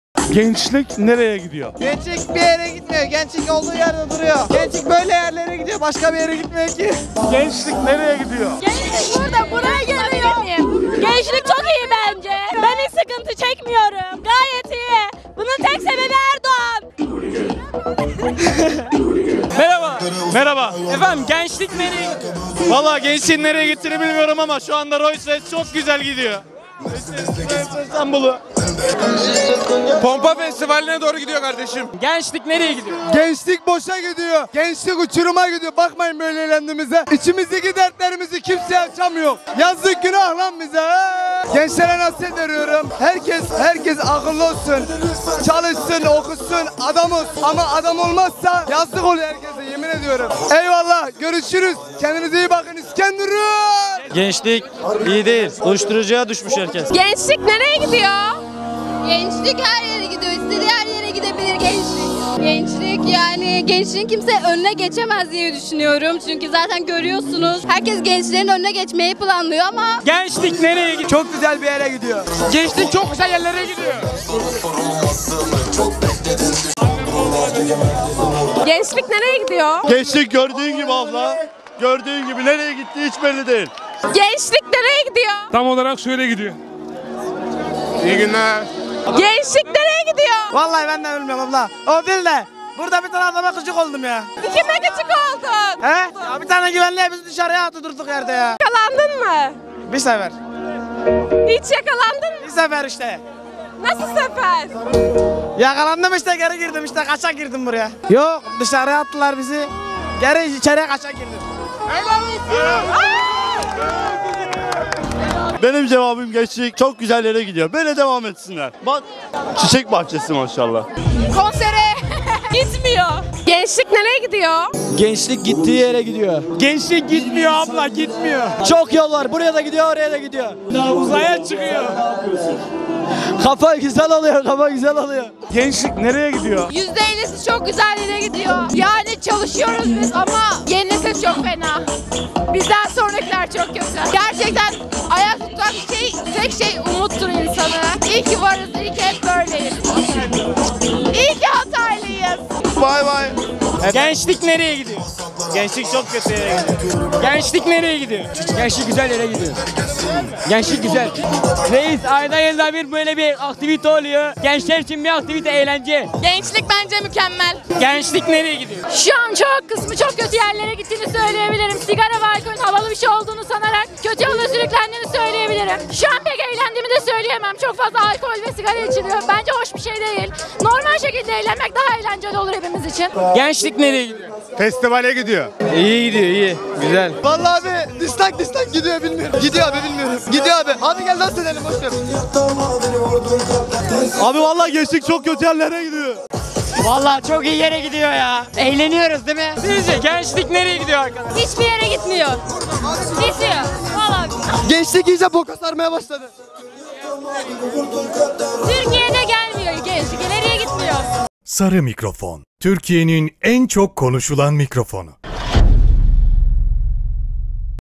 Gençlik Nereye Gidiyor? Festivalin Nabzını Tuttuk!
Bu röportajda, gençlerin dünyasını daha yakından tanımak için festivalde mikrofonu uzattık. "Gençlik nereye gidiyor?" sorusunu sorduk ve farklı bakış açılarıyla gençlerin gelecekten beklentilerini, hayallerini ve karşılaştıkları zorlukları dinledik. Gelecek nesillerin sesi olmak için bizi izlemeye devam edin!